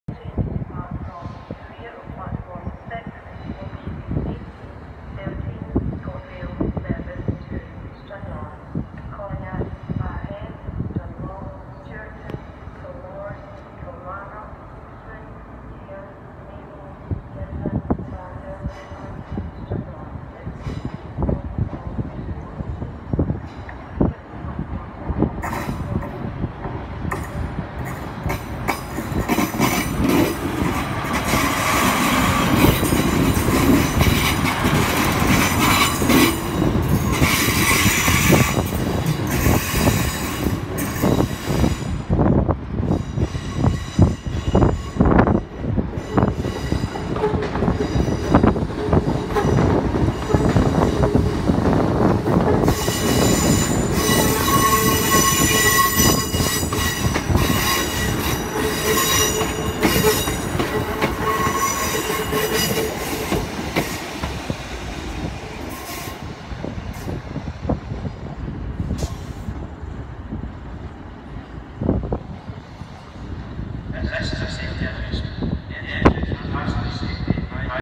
Glasgow Central Trains
A short videos of trains at Glasgow Central on 7th June 2017 showing two passing 2 car Class 156 with a Class 314 behind them.